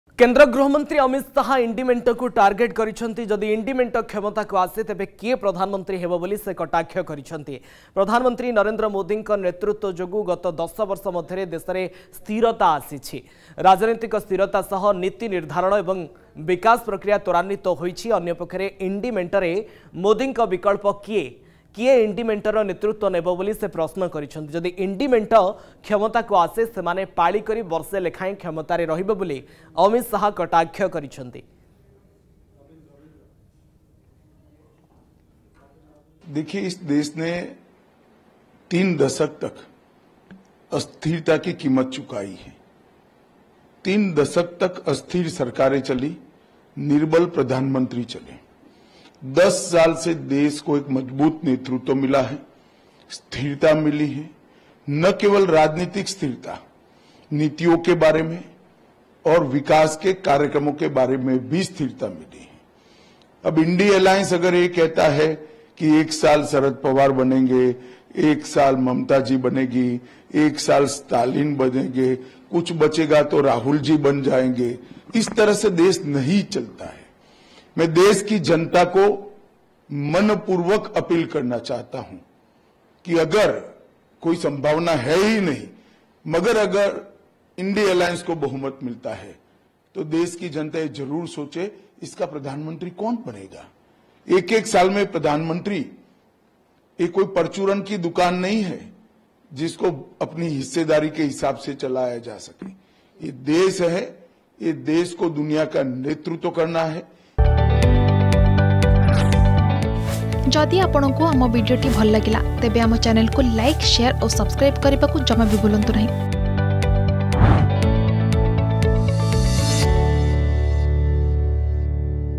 ଇଣ୍ଡିମେଣ୍ଟକୁ ଶାହଙ୍କ ଟାର୍ଗେଟ୍ || Amit Shah
କେନ୍ଦ୍ର ଗୃହମନ୍ତ୍ରୀ ଅମିତ ଶାହ ଇଣ୍ଡି ମେଣ୍ଟକୁ ଟାର୍ଗେଟ୍ କରିଛନ୍ତି । ଯଦି ଇଣ୍ଡିମେଣ୍ଟ କ୍ଷମତାକୁ ଆସେ ତେବେ କିଏ ପ୍ରଧାନମନ୍ତ୍ରୀ ହେବ ବୋଲି ସେ କଟାକ୍ଷ କରିଛନ୍ତି । ପ୍ରଧାନମନ୍ତ୍ରୀ ନରେନ୍ଦ୍ର ମୋଦିଙ୍କ ନେତୃତ୍ବ ଯୋଗୁଁ ଗତ ୧୦ ବର୍ଷ ମଧ୍ୟରେ ଦେଶରେ ସ୍ଥିରତା ଆସିଛି ।